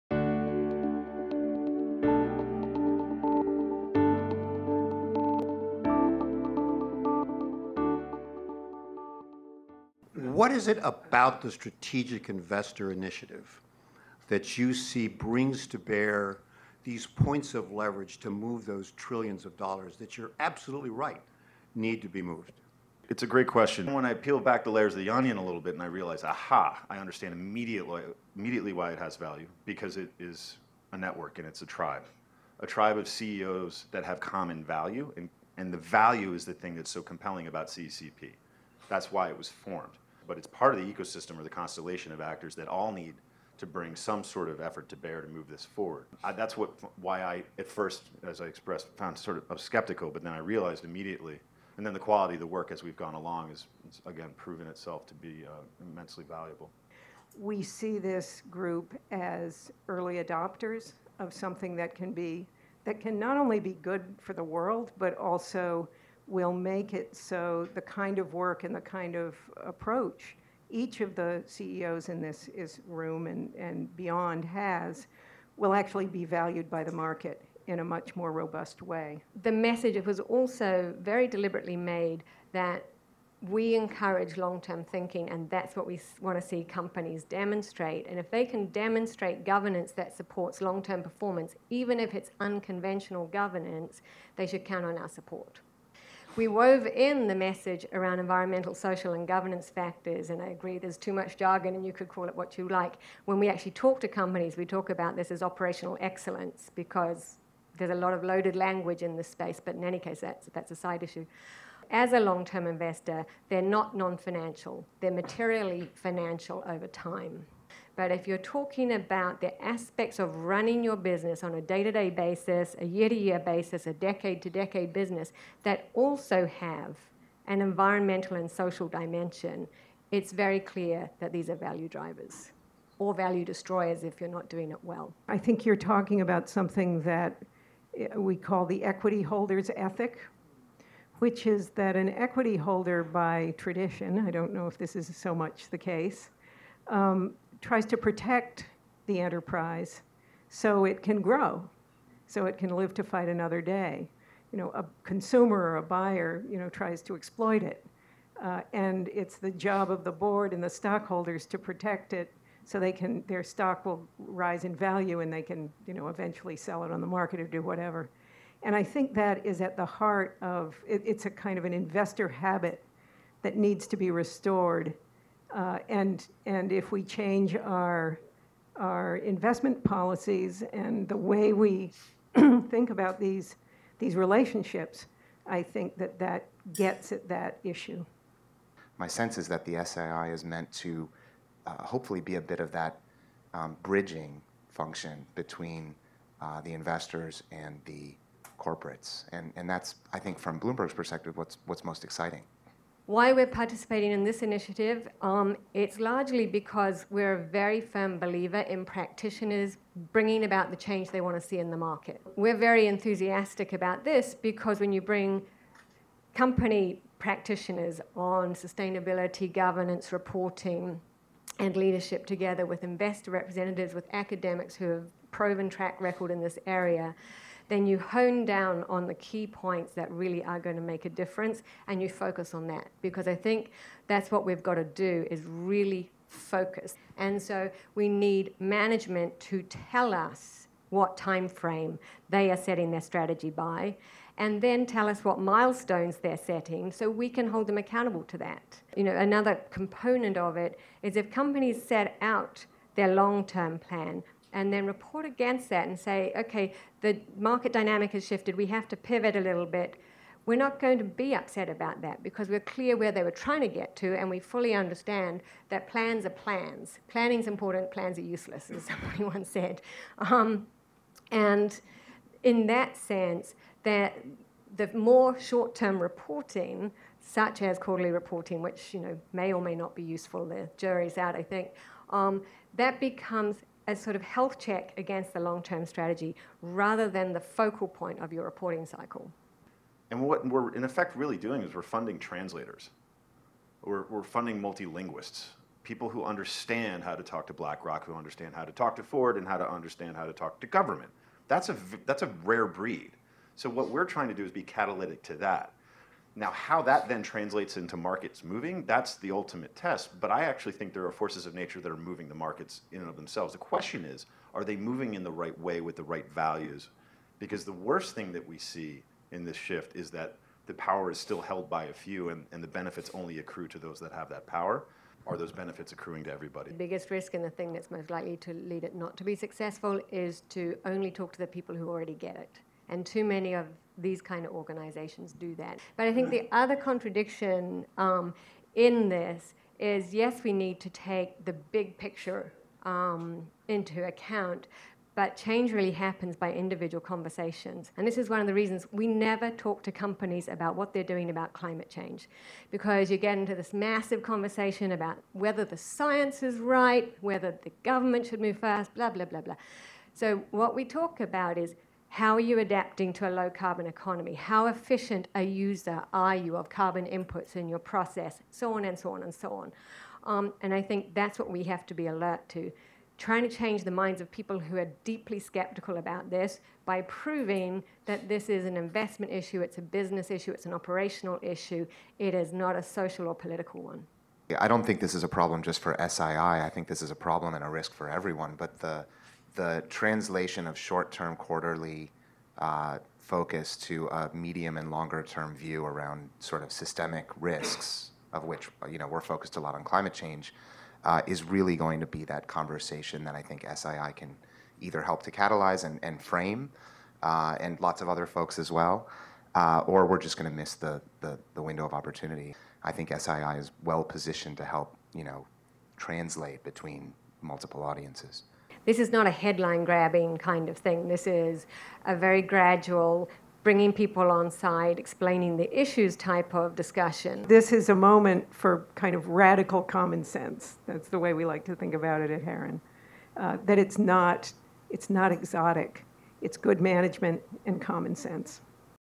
Strategic Investor Initiative Briefing at Bloomberg, September 22, 2016
On September 22, 2016, investors were invited to attend a panel on the future of long-termism as the gateway to ESG integration in capital markets and learn more about the Strategic Investor Initiative's strategy, activites, and engagement opportunities.